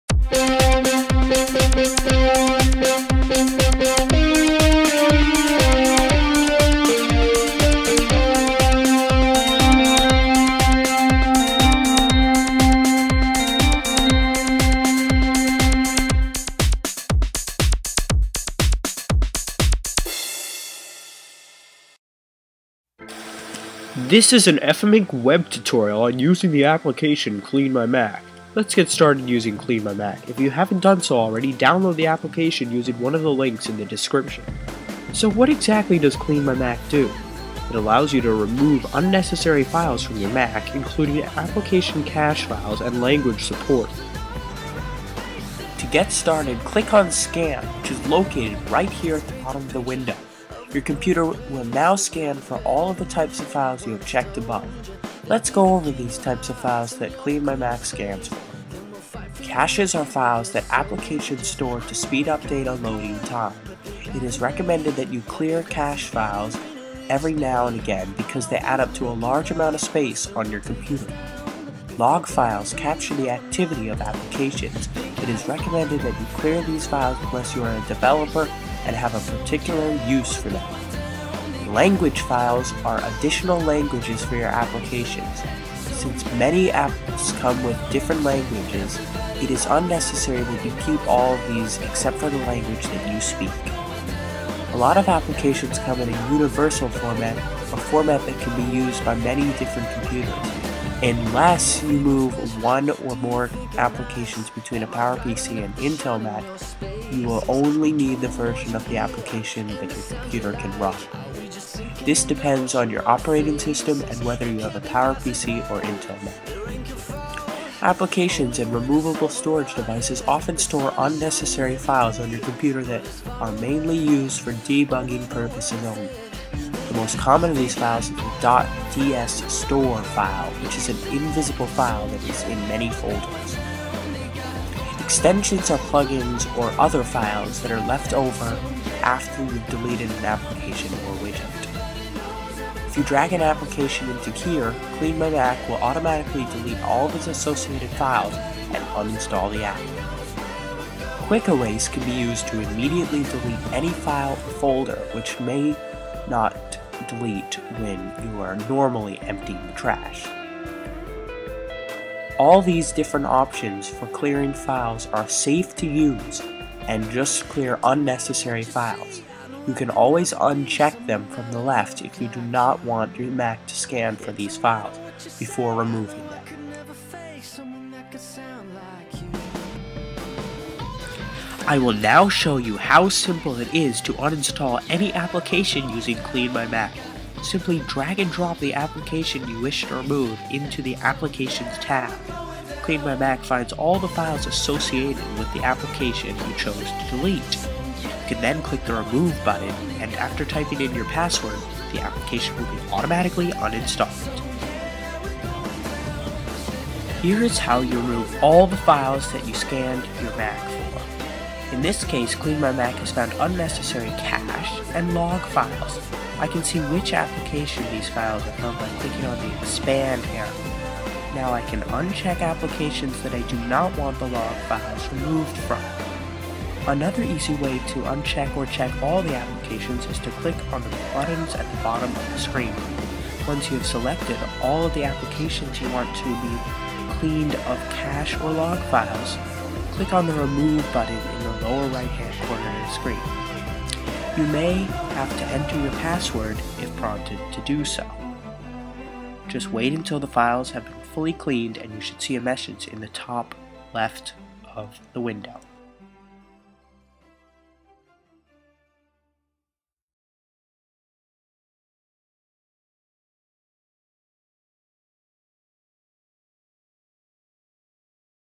CleanMyMac - Tutorial
This is an FM Inc. Web tutorial on using the application CleanMyMac.
CleanMyMac allows you to remove unnecessary files from your Mac including application cache files and language support. In addition, it also is able to uninstall any application by removing all of its associated files.